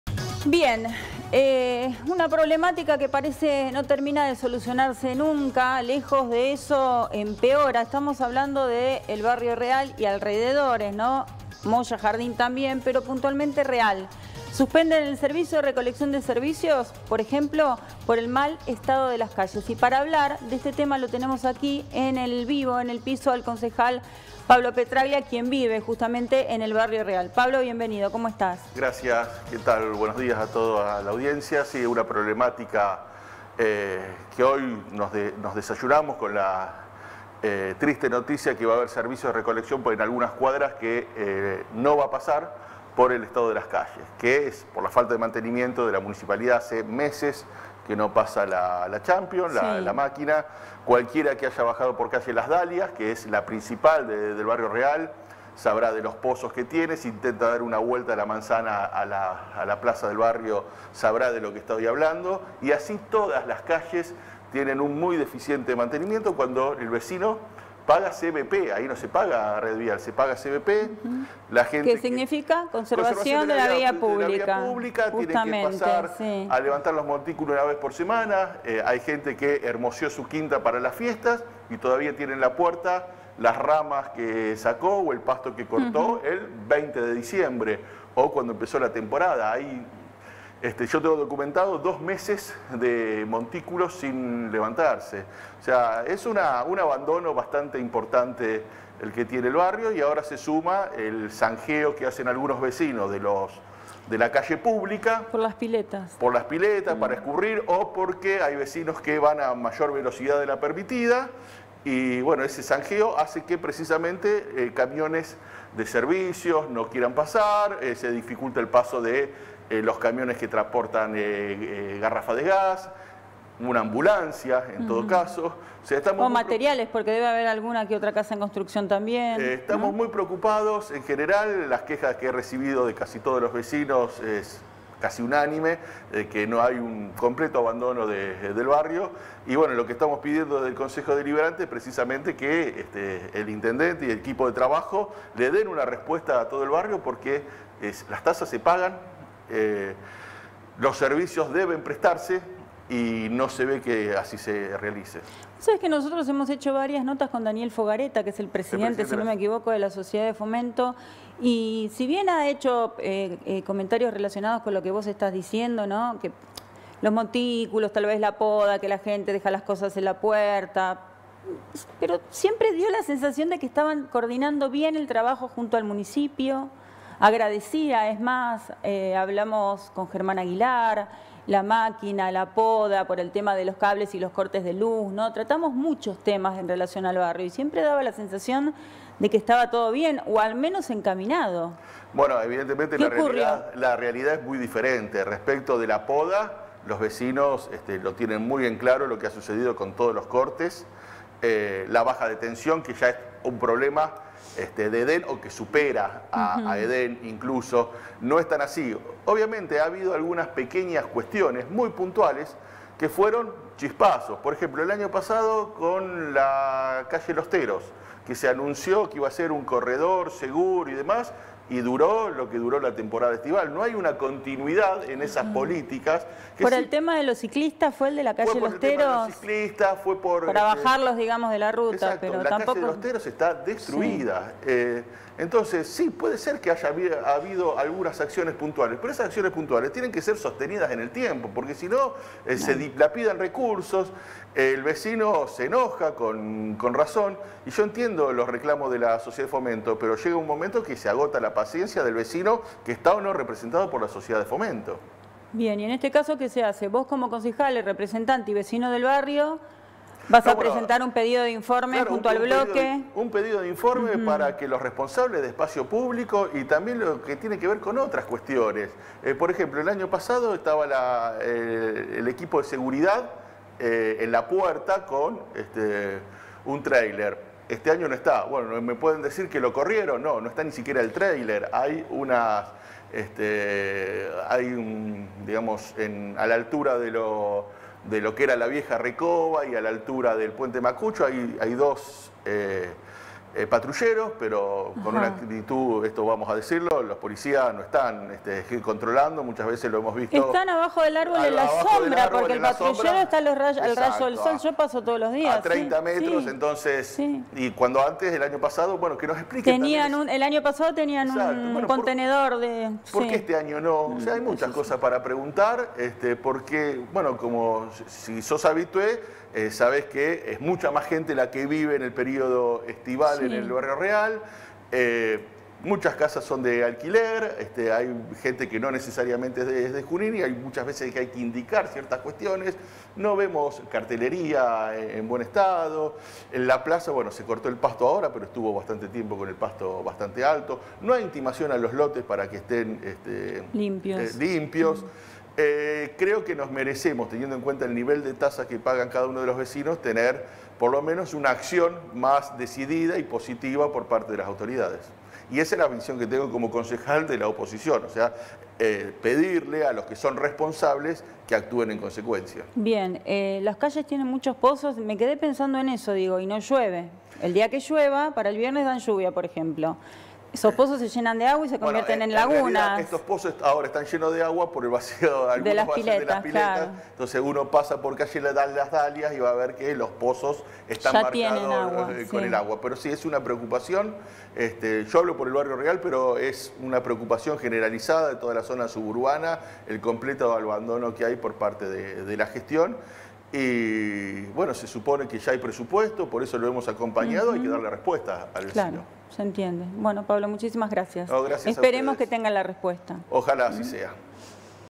Reclamo: Pastizales y calles en mal estado en barrio Real
Pablo Petraglia, concejal FDT, en 10Noticias

🖥 Encontrá más noticias y toda la programación en nuestra web →